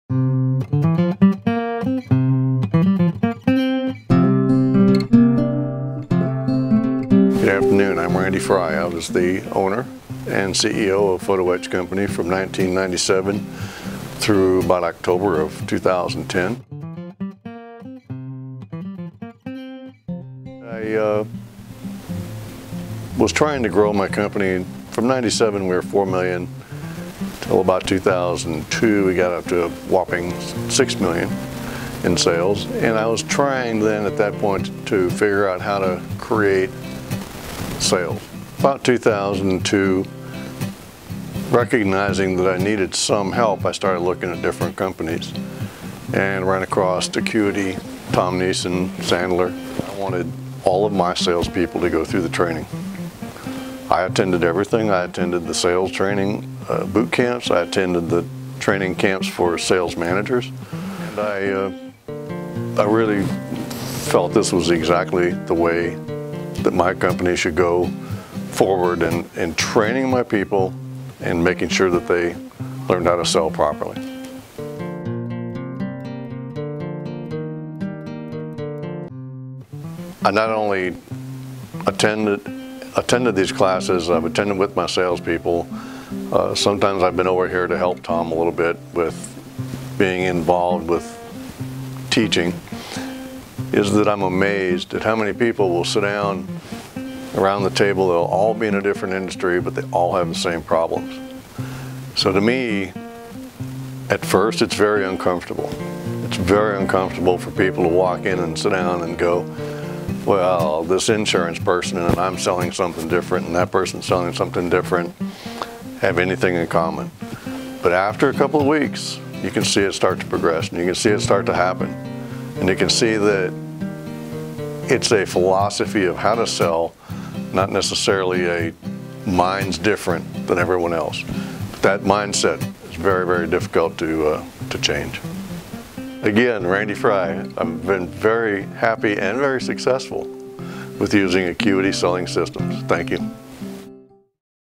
Randy Fry Testimonial
Hear what Randy Fry has to say about Acuity Systems, Inc.!

Check him out on linkedIn!